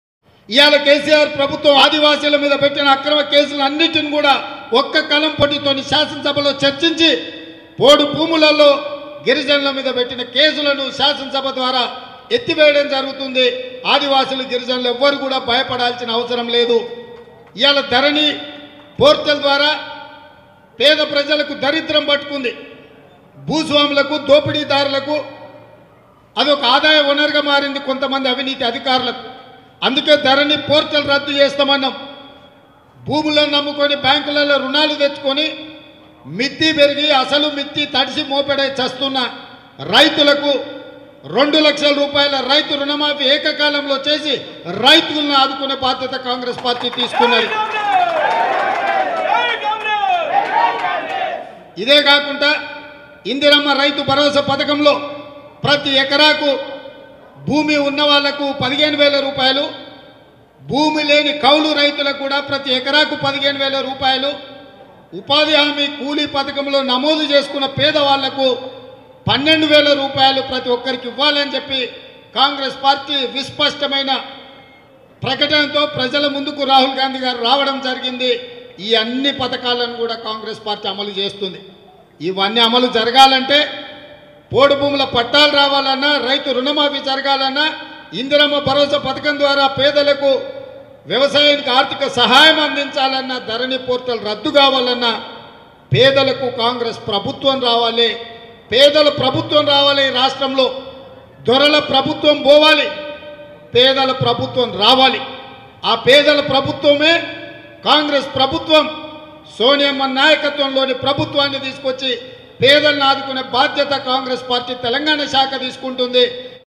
అక్రమ కేసులను ఎత్తివేస్తాం
తెలంగాణలో 2023 ఎన్నికలలో కాంగ్రెస్ ప్రభుత్వం అధికారంలోకి వస్తుందని తెలంగాణ ప్రదేశ్ కాంగ్రెస్ కమిటీ అధ్యక్షుడు రేవంత్ రెడ్డి అన్నారు. తమ పార్టీ అధికారంలోకి టిఆర్ఎస్ ప్రభుత్వం ఆదివాసీలపై అక్రమంగా పెట్టిన కేసులను ఎత్తివేస్తామని ధరణి పోర్టల్ రద్దు చేస్తామని రుణమాఫీ చేస్తామని అన్నారు.